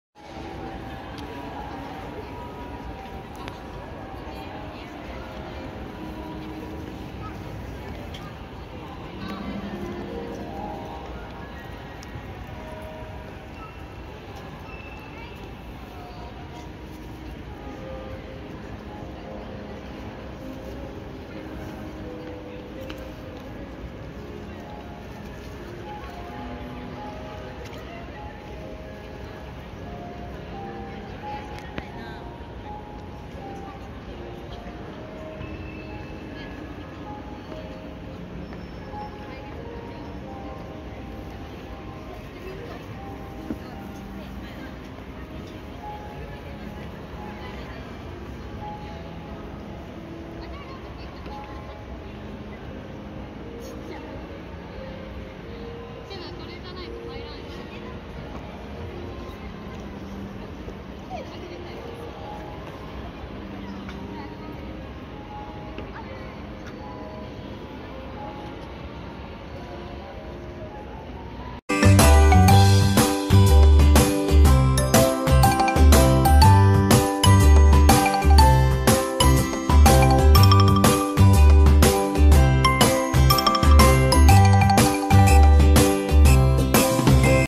【京都駅ビルクリスマスイルミネーション2018】22メートル巨大クリスマスツリー【京都駅ビル4階室町小路広場】（2018/11/26）
京都駅ビルクリスマスイルミネーション2018
4階室町小路広場にある「22メートルキョダイクリスマスツリー」です。

キレイでした。